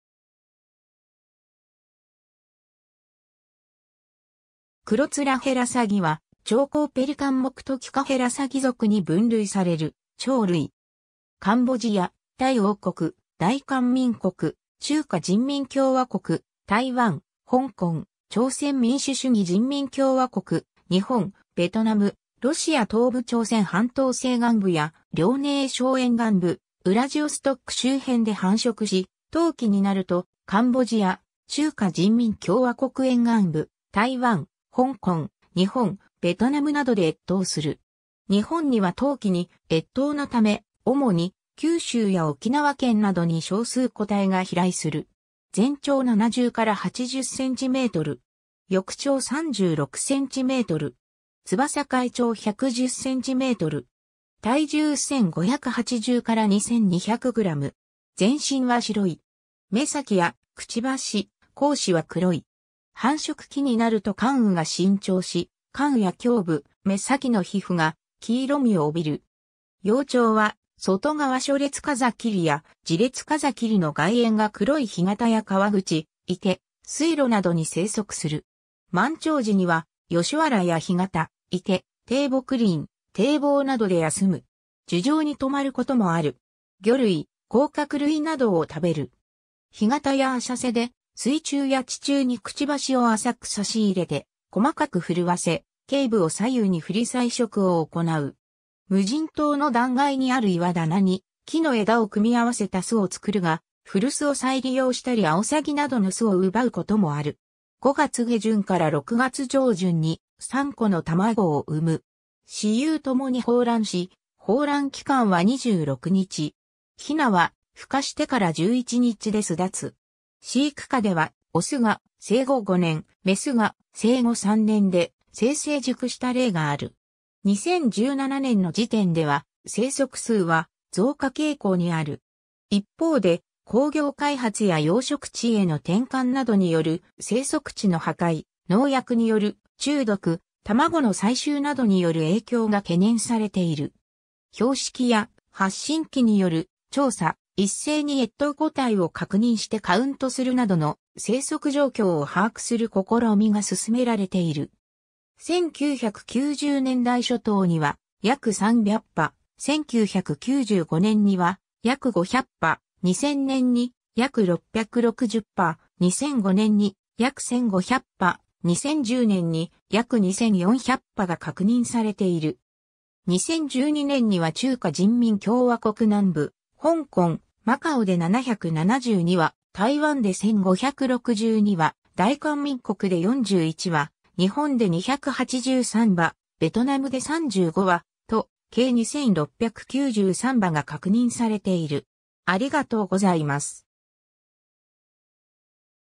クロツラヘラサギ
クロツラヘラサギ（黒面箆鷺、Platalea minor）は、鳥綱ペリカン目トキ科ヘラサギ属に分類される鳥類。
 カンボジア、タイ王国、大韓民国、中華人民共和国、台湾、香港、朝鮮民主主義人民共和国、日本、ベトナム、ロシア東部 朝鮮半島西岸部や遼寧省沿岸部・ウラジオストック周辺で繁殖し、冬季になるとカンボジア・中華人民共和国沿岸部・台湾・香港・日本・ベトナムなどで越冬する。
日本には冬季に越冬のため、主に九州や沖縄県などに少数個体が飛来する（冬鳥）。
翼長36センチメートル。
翼開長110センチメートル。
体重1,580 - 2,200グラム。
全身は白い。
 眼先や嘴、後肢は黒い。
繁殖期になると冠羽が伸長し、冠羽や胸部・眼先の皮膚が黄色みをおびる。
幼鳥は外側初列風切や次列風切の外縁（羽縁）が黒い 干潟や河口・池・水路などに生息する。
満潮時にはヨシ原や干潟・池・低木林・堤防などで休む。
樹上にとまることもある。
 魚類、甲殻類などを食べる。
干潟や浅瀬で水中や地中に嘴を浅く差し入れて細かくふるわせ、頸部を左右に振り採食を行う。
 無人島の断崖にある岩棚に木の枝を組み合わせた巣をつくるが、古巣を再利用したりアオサギなどの巣を奪うこともある。
5月下旬から6月上旬に、3個の卵を産む。
雌雄共に抱卵し、抱卵期間は26日。
雛は孵化してから11日で巣立つ。
飼育下ではオスが生後5年、メスが生後3年で性成熟した例がある。
 2017年の時点では生息数は増加傾向にある。
一方で工業開発や養殖池への転換などによる生息地の破壊、農薬による中毒、卵の採集などによる影響が懸念されている。
標識や発信機による調査、一斉に越冬個体を確認してカウントするなどの生息状況を把握する試みが進められている。
1990年代初頭には約300羽、1995年には約500羽、2000年に約660羽、2005年に約1,500羽、2010年に約2,400羽が確認されている。
2012年には中華人民共和国南部・香港・マカオで772羽、台湾で1,562羽、大韓民国で41羽、日本で283羽（県別の内訳は後述）、ベトナムで35羽、と計2,693羽が確認されている。